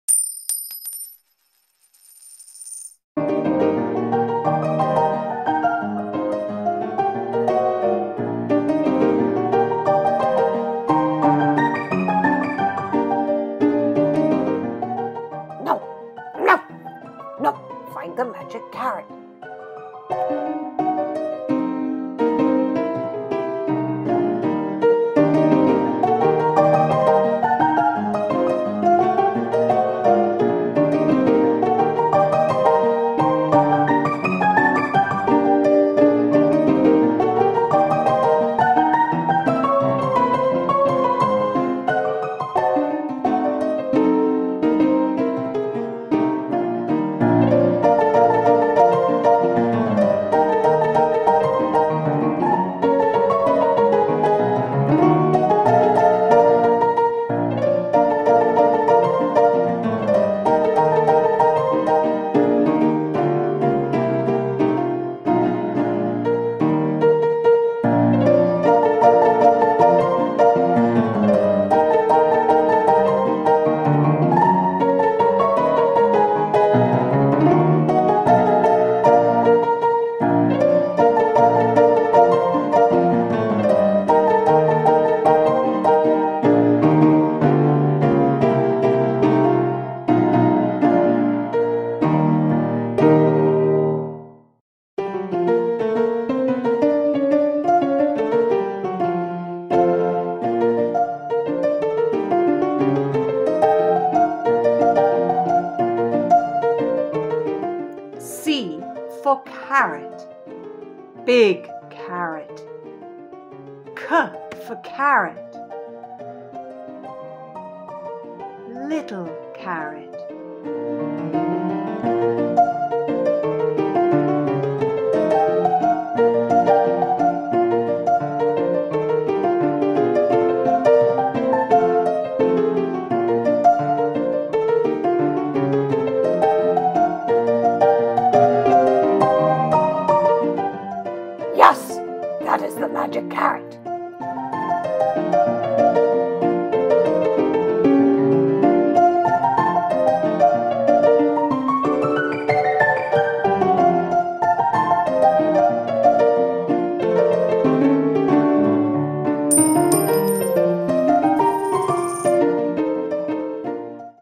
C for Carrot - Carrots are Yummy - Ms. Bits N' Bobs and Her Magical Cloak - ABC series
C for Carrot - Ms. Bits N' Bobs and Her Magical Cloak - ABC series

Cuddle in and learn all about G for Giraffe with Ms. Bits N' Bobs and Her Magical Cloak.  ABC series